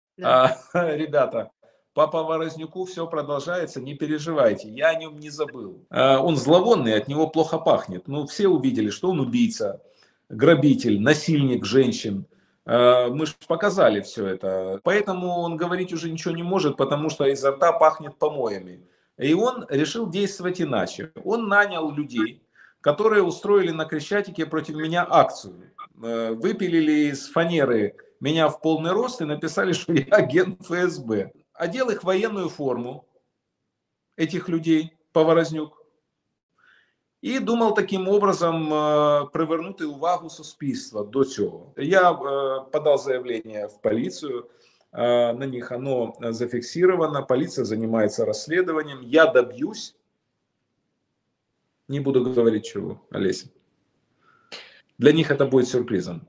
Гордон: Поворознюк устроил провокацию с псевдовоенными
Фрагмент совместного стрима Алеси Бацман и Дмитрия Гордона.

Перевод в евро можно сделать здесь:
HORDON DMYTRO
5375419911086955
Swift code: UNJSUAUKXXX
Kyiv, Ukraine